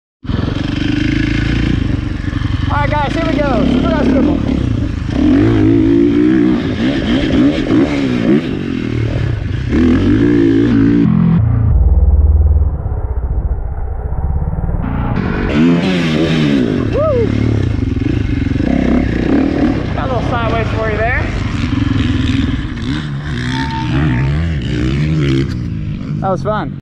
How fast do you have to go to jump a Supercross Triple!?
I have always wondered the speed it takes to jump a triple. Now I know!
30 MPH will get you there in this scenario. 😎